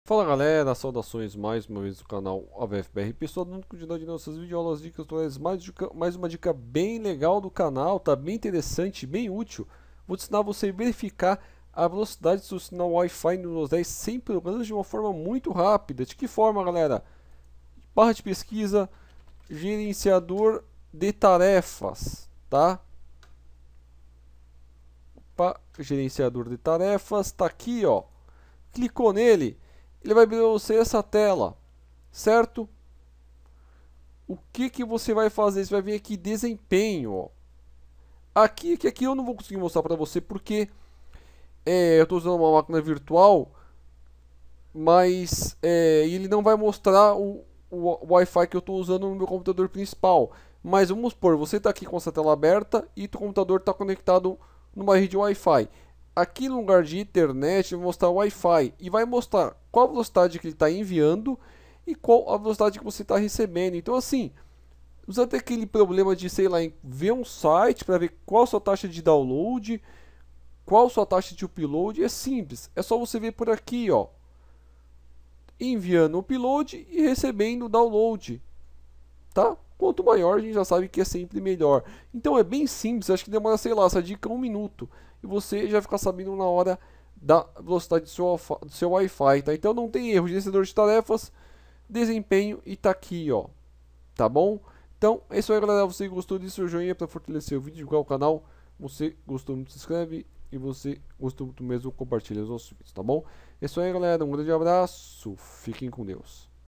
Aprenda a Verificar a Velocidade Do Seu Sinal Wifi No Windows 10 Sem Programas
Fala galera !! Segue mais outra dica bem legal, aprendam a verificar a velocidade do seu sinal Wifi no Windows 10 sem programas e facilmente.